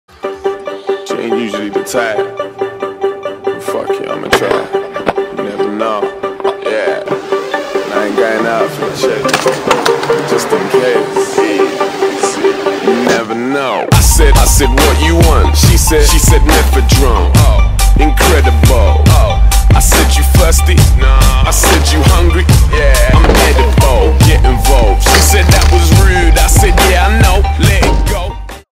James Astbury - Tres Clips
filmed in one night and james absolutely killed it, his riding just shouts  out style and here it is for people to see